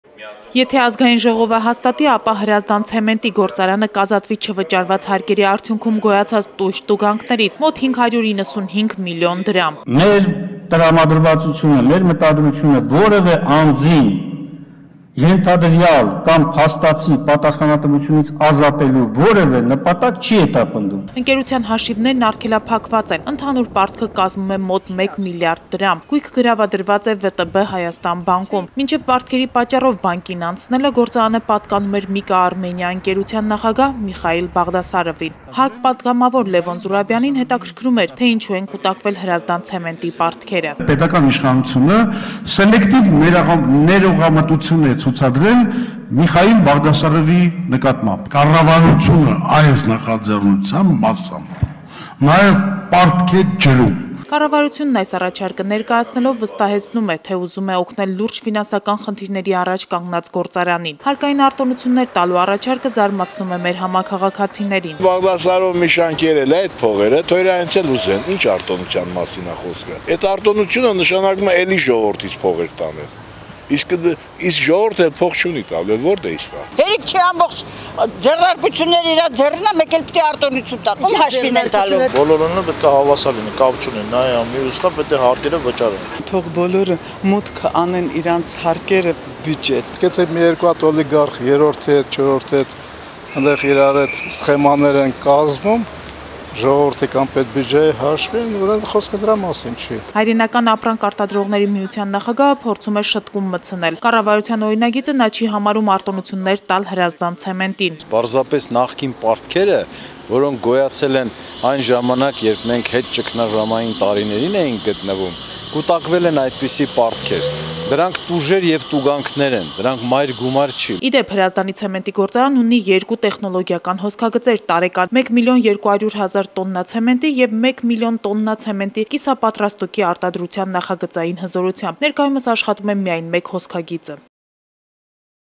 «Բաղդասարով Միշան կերել է այդ փողերը, թող իրենից էլ ուզեն». հարցում
Եթե  Աժ-ն հաստատի, ապա  Հրազդանի ցեմենտի գործարանը կազատվի  չվճարված հարկերի  արդյունքում գոյացած  տույժ –տուգանքներից՝  մոտ 595  միլիոն դրամ։

Ընկերության հաշիվները արգելափակված են, ընդհանուր պարտքը կազմում է մոտ մեկ միլիարդ դրամ, գույքը գրավադրված է «ՎՏԲ Հայաստան» բանկում։  Մինչեւ պարտքերի պատճառով բանկին անցնելը,  գործարանը  պատկանում  էր MIKA Armenia ընկերության նախագահ Միխայիլ Բաղդասարովին: Հրազդանի ցեմենտի գործարանն ունի ունի երկու տեխնոլոգիական հոսքագծեր` տարեկան 1,2 մլն տոննա ցեմենտի և 1 մլն տոննա ցեմենտի կիսապատրաստուկի արտադրության նախագծային հզորությամբ։ Ներկայումս աշխատում է միայն մեկ հոսքագիծը։